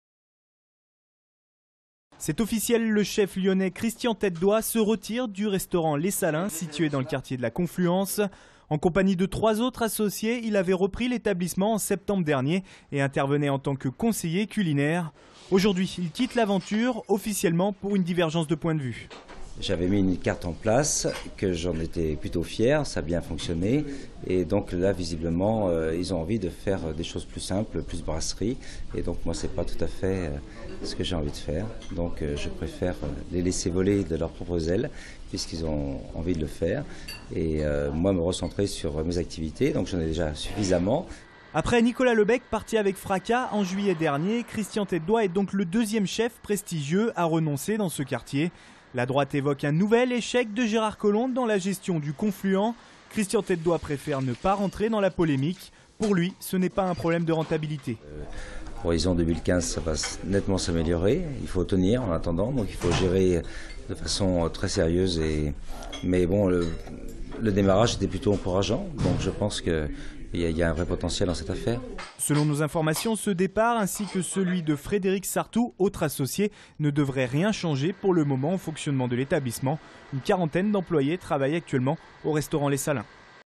Confluence: Christian Têtedoie jette l'éponge
Sites webs/blogs, copiez cette vidéo avec le bouton "Intégrer" !
 C'est officiel, Christian Têtedoie se retire du restaurant Les Salins situé dans le quartier de la Confluence.